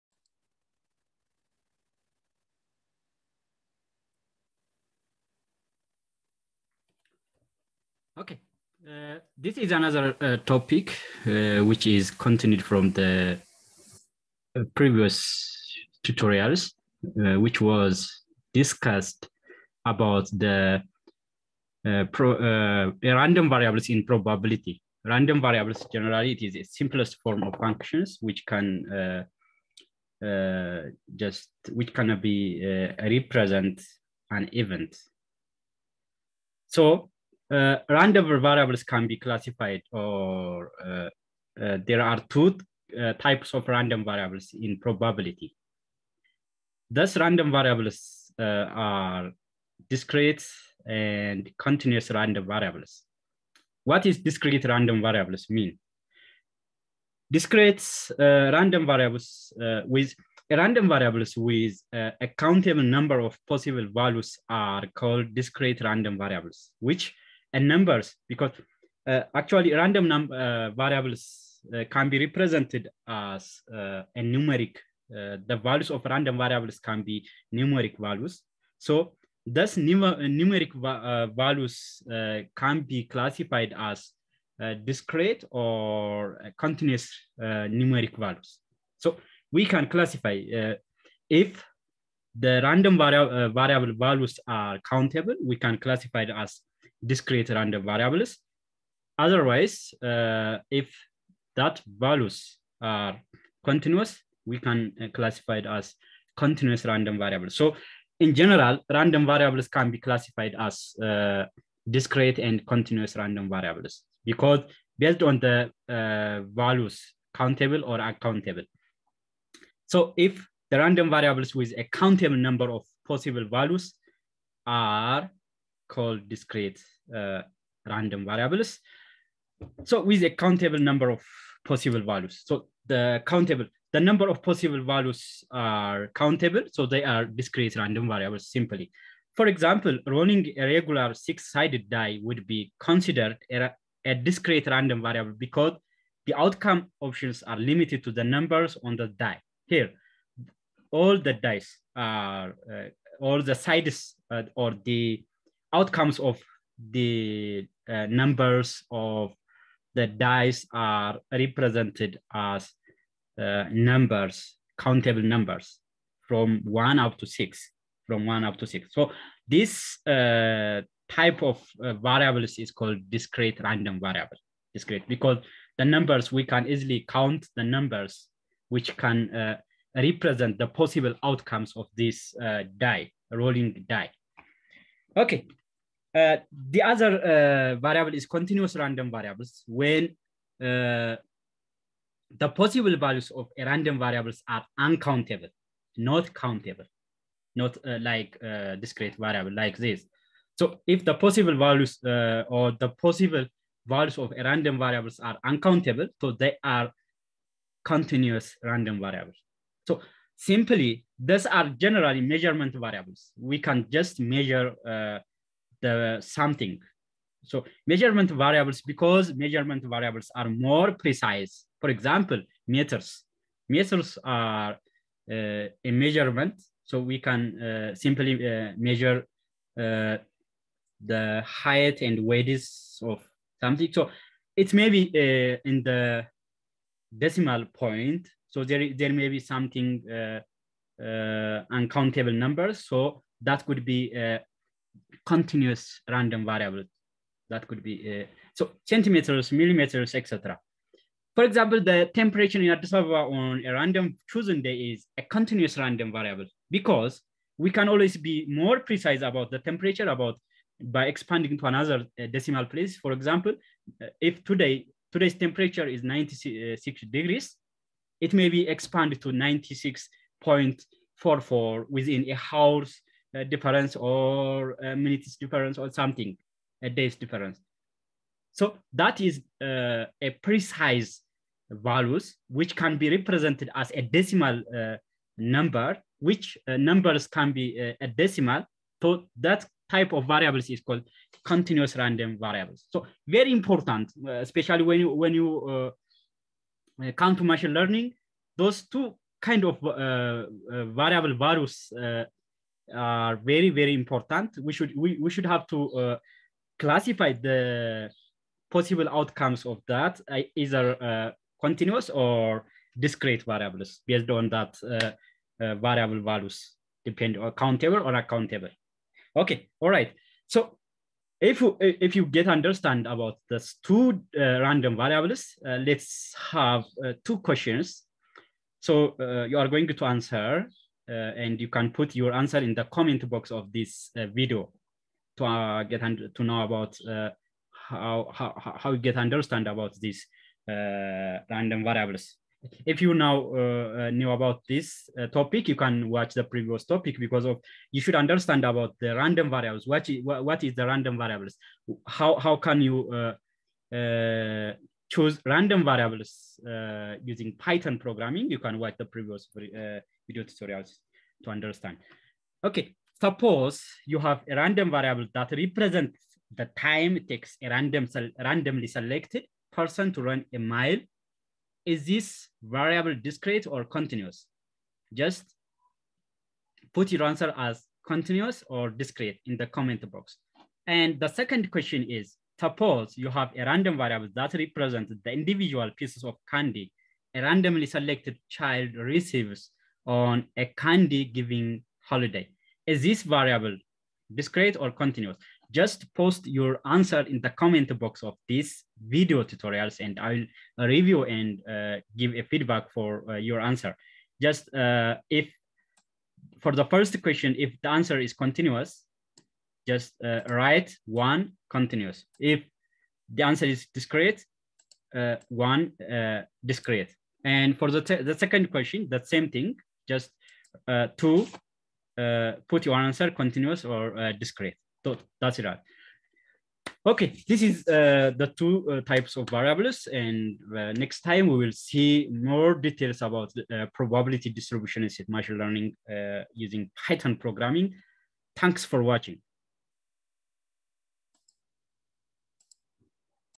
Introduction to Probability Distribution for Machine Learning | Discrete or Continuous
Learn about the discrete and continuous random variable